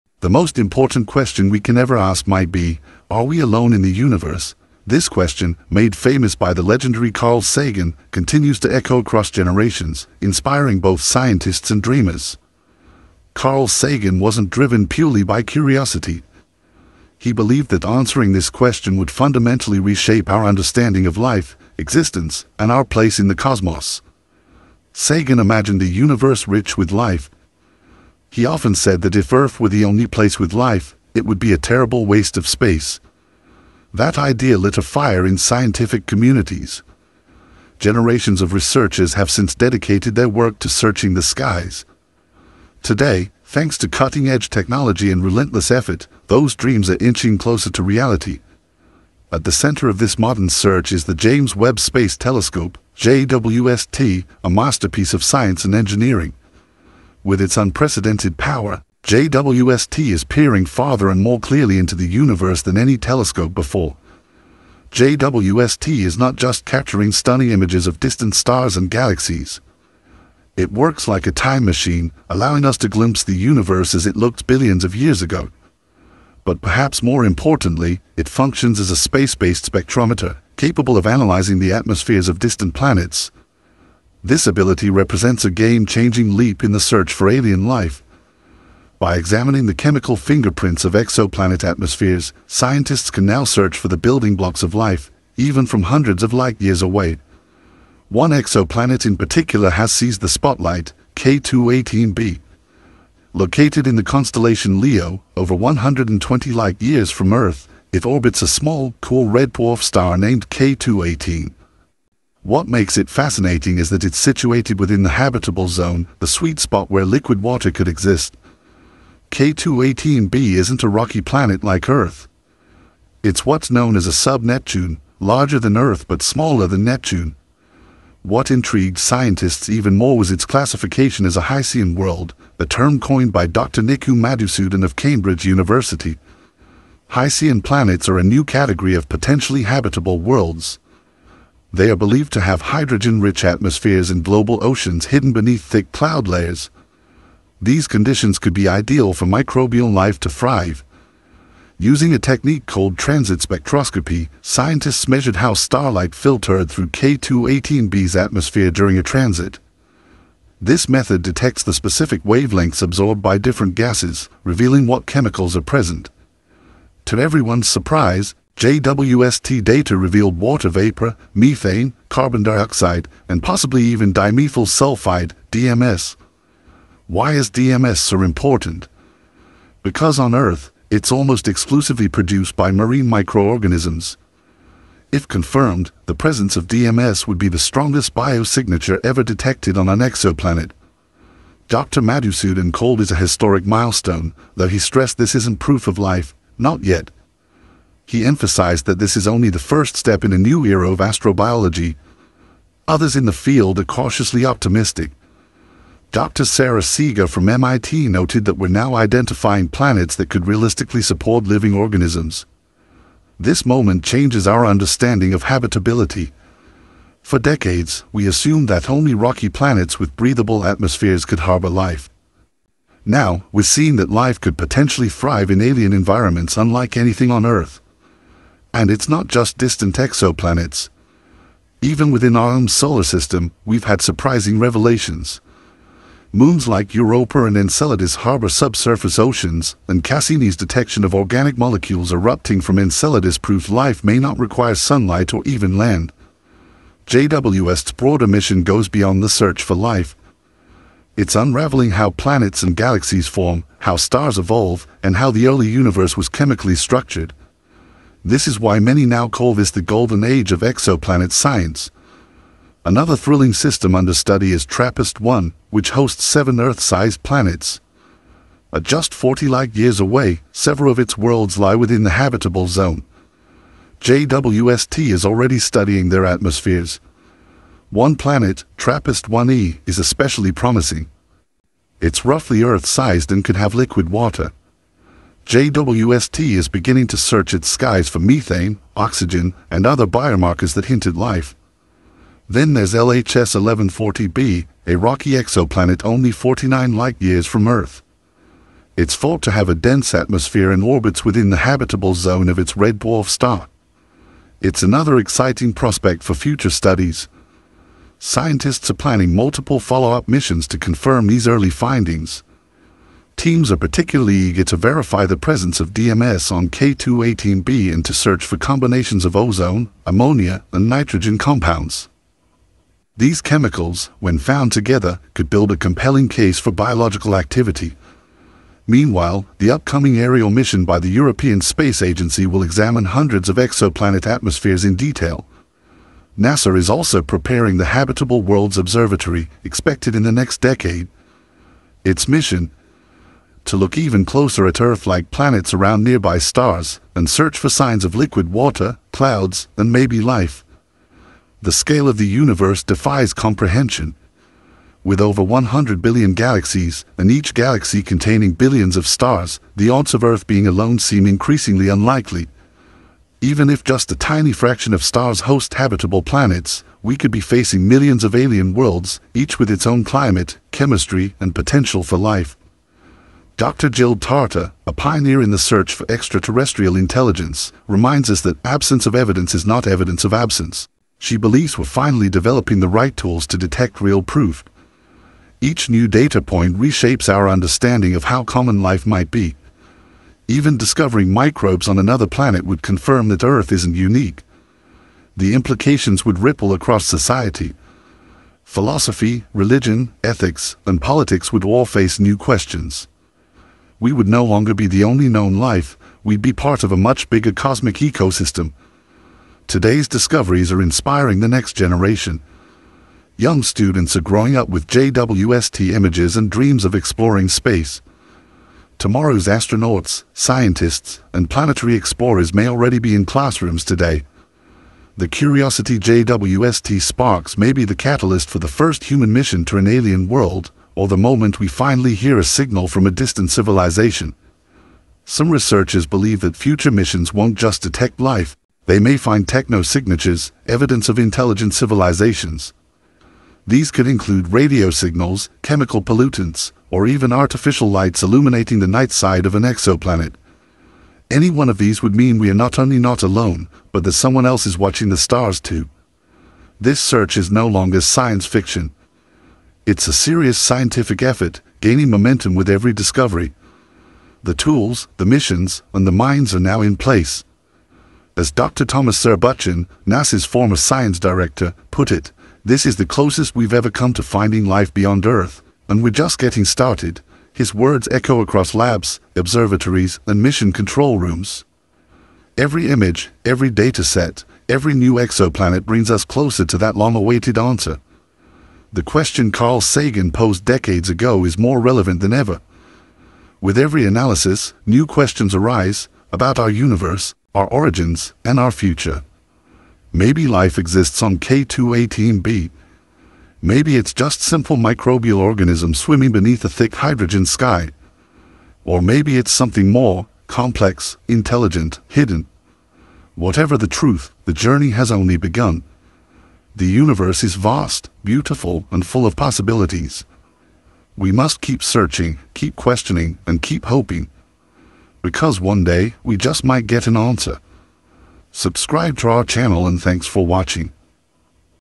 James Webb Telescope Announces First Real Image of Exoplanet K2-12B!
Join us on an exciting journey to explore the mysterious exoplanet K2-18b 🌌.
 Discover how the James Webb Space Telescope is unveiling secrets about this distant world,
 including the presence of water vapor and potential signs of life. Could this be the breakthrough we've been waiting for? 🪐